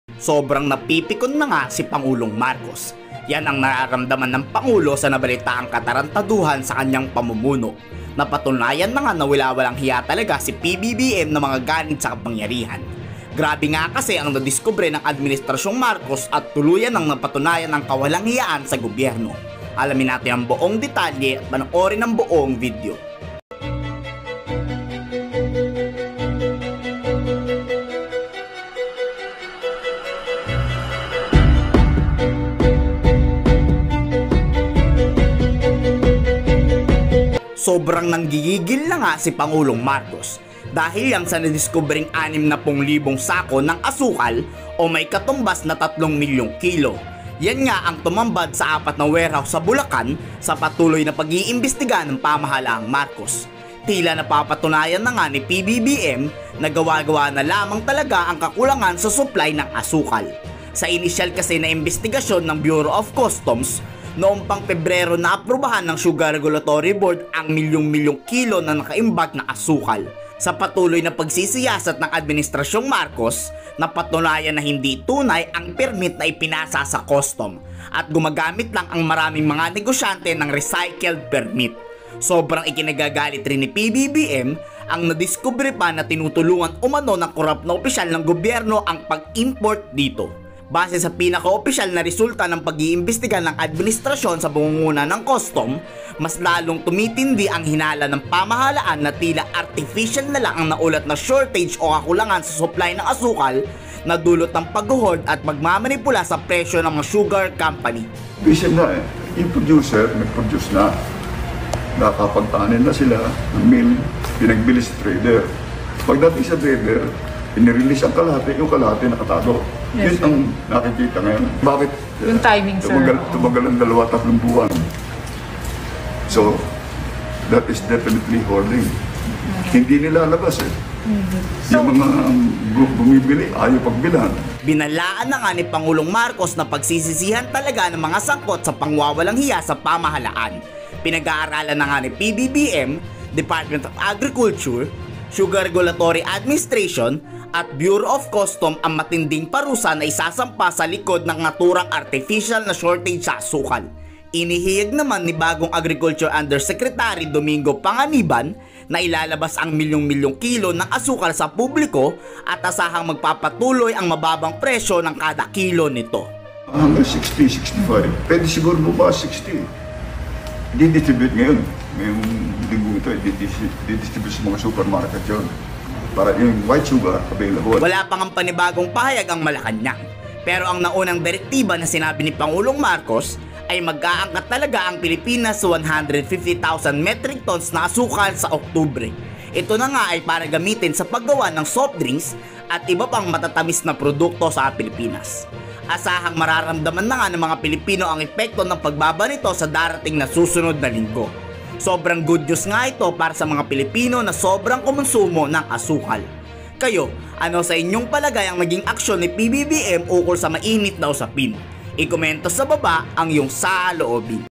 PRES MARCOS! NAKAKA BIGLA! NAG BABALA NA! MAS LALONG NAKAKATAKOT SI PBBM! G NA G SA NALAMAN!
Sobrang ikinagalit ni Pangulong Ferdinand Marcos ang NADISKUBRE ng kanyang PAMAHALAAN.

Nang gigigil ng sobra sa galit ang PANGULO sa nalaman.